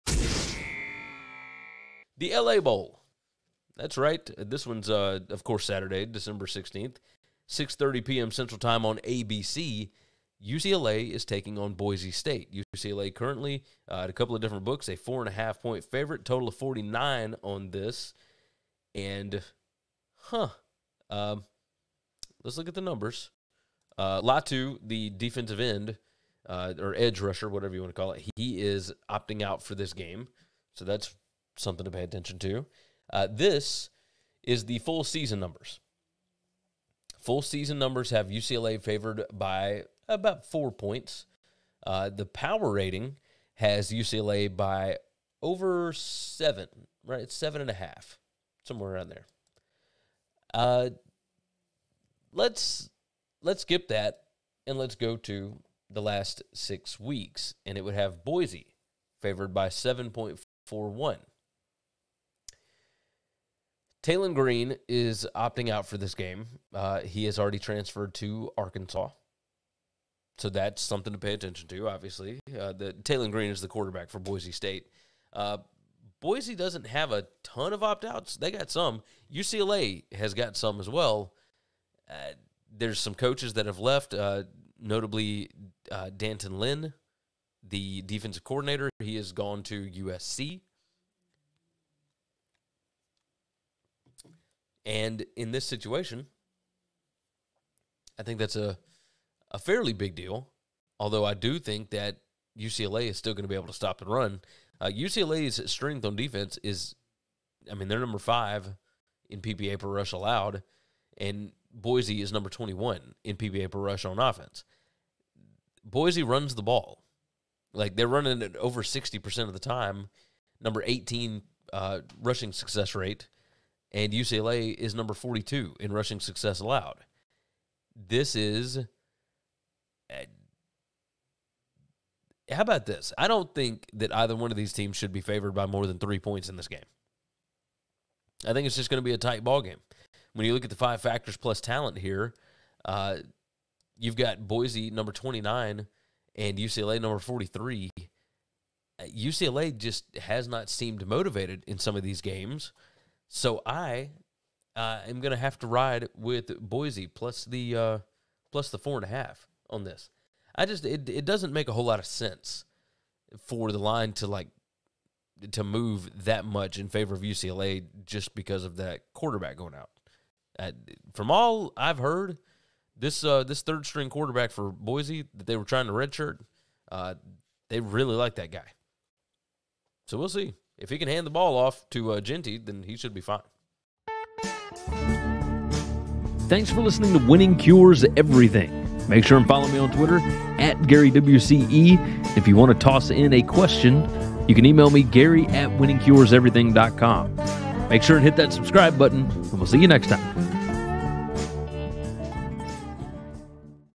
LA Bowl: UCLA vs Boise State Picks & Prediction Against the Spread 2023
LA Bowl: UCLA vs Boise State Advanced Stats Picks Against the Spread 2023 Predictions

Gary gives a quick preview, analysis, score prediction based on advanced stats and data & his pick (lean) against the spread (ATS) for the 2023 LA Bowl matchup featuring Boise State vs UCLA!

 🕊️ Instagram / TikTok : @GaryWCE  |  Twitter: @WinningCures